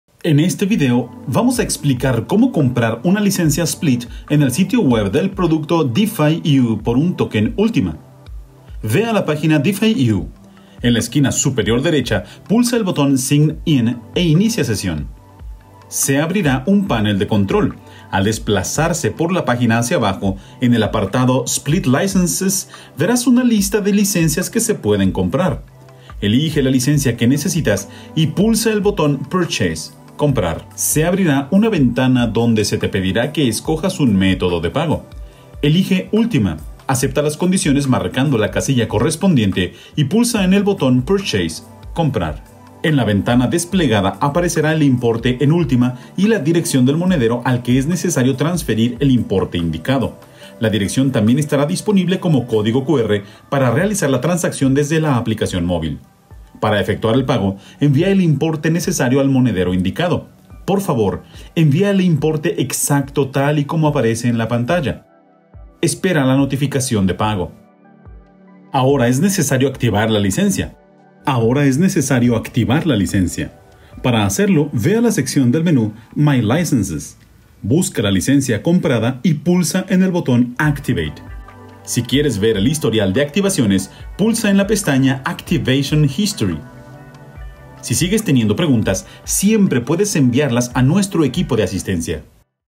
¿Cómo comprar una licencia split?
En este tutorial, te explicaremos cómo comprar una licencia SPLIT en el sitio de productos DeFi-U a cambio de un token Ultima.